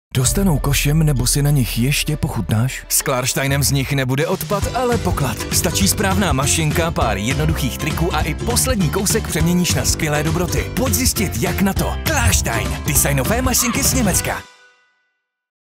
Žádný odpad, pouze poklad! | Klarstein Česká republika
Dovolíte, aby se ze zbytků stal odpad, nebo z nich raději uděláte poklad? Naučte se spolu s námi jak využít každou potravinu, surovinu a mašinku naplno. Máme pro vás připravené skvělé tipy a triky do domácnosti, návody jak využít i slupky nebo zbytky jídel a připravit z nich skvělé dobroty, ale hlavně vám ukážeme, co všechno dokážou mašinky, které už máte doma!